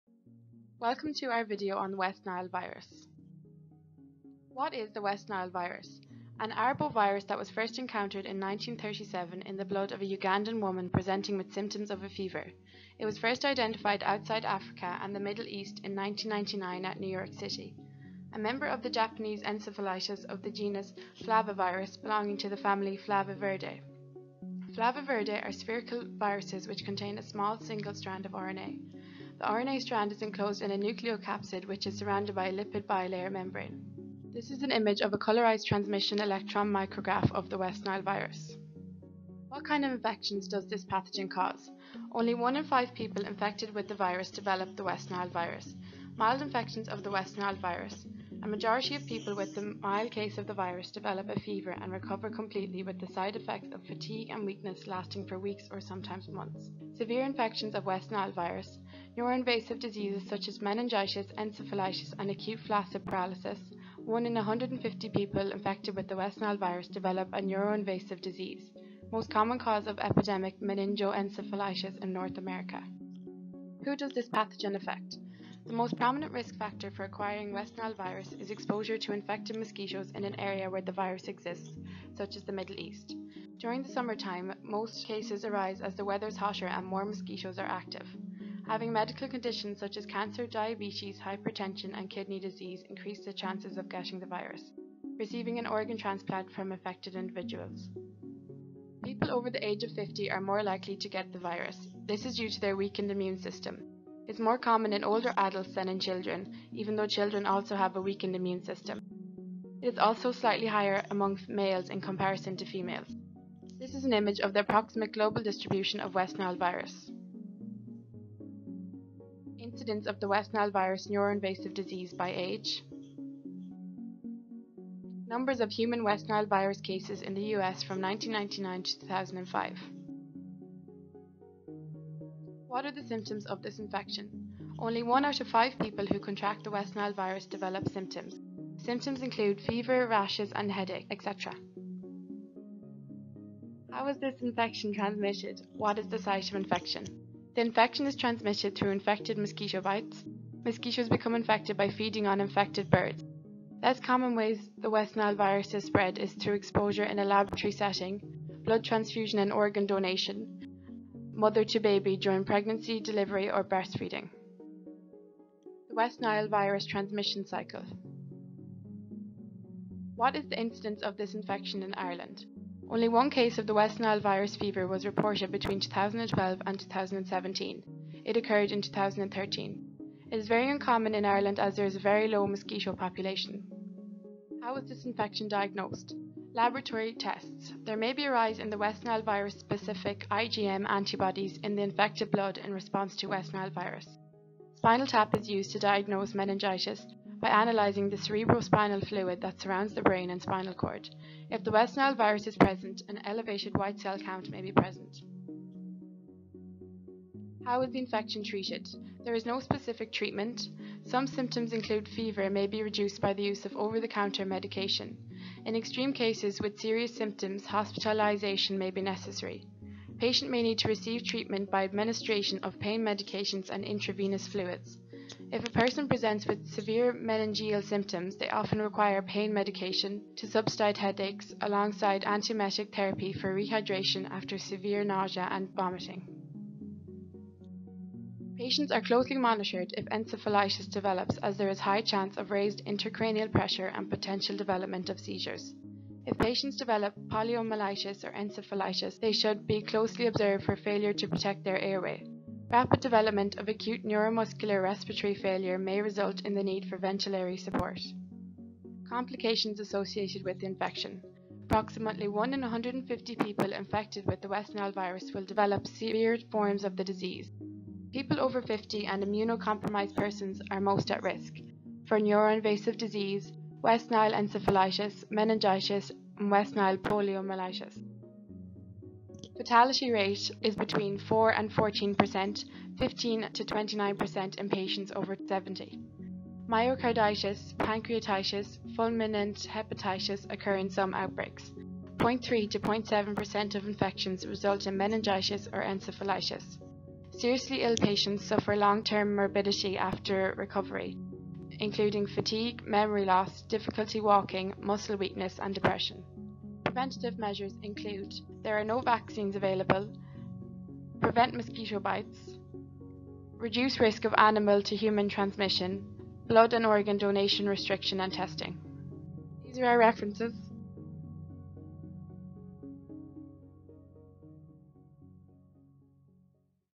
West Nile Virus
This is a video produced by a group of Year 2 Medical Science students at GMIT on the West Nile Virus.

Credits:
Research: Laoise Naughton, Lisa Brogan, Myan Yan Leng , Zara Brady
Voice-over: Laoise Naughton
Editing: Myan Yan Leng